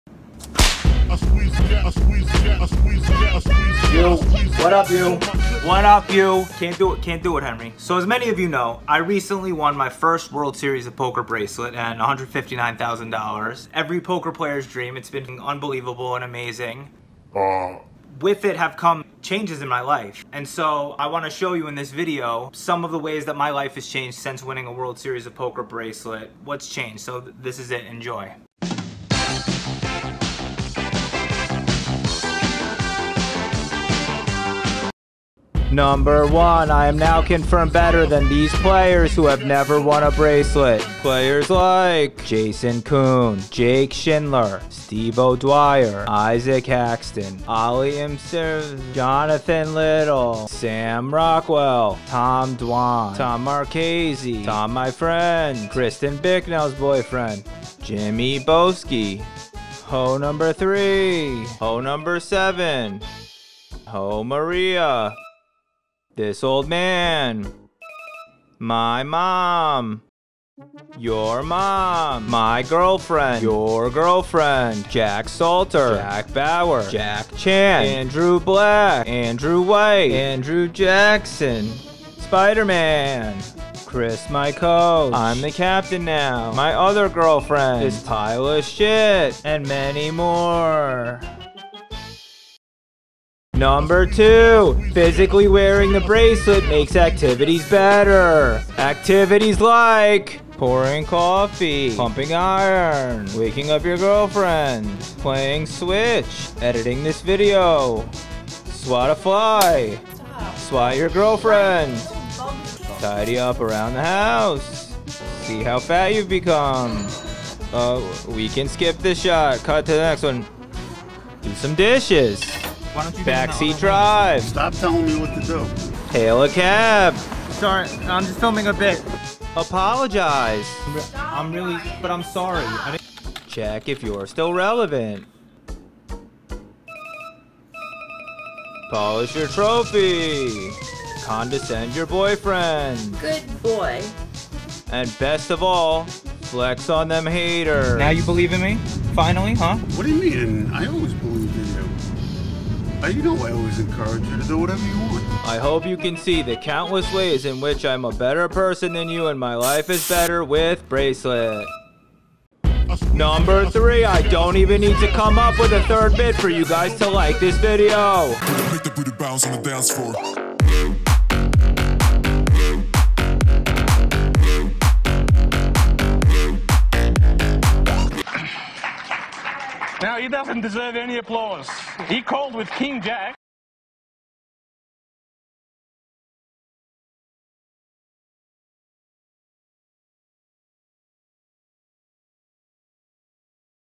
The Perks of Winning a WSOP Bracelet
I recently won a WSOP Bracelet and $159K as many of you know. Here is how it has changed my life. In this weeks poker vlog and gambling vlog I go over what is new in my life since winning world series of poker bracelet in 2020

VIDEO EDITING & GRAPHICS BY.
    ME BRO I EDITED THIS ONE

BUT ON OTHER VIDEOS AND IN GENERAL I AM HELPED BY
JOHN MALONE: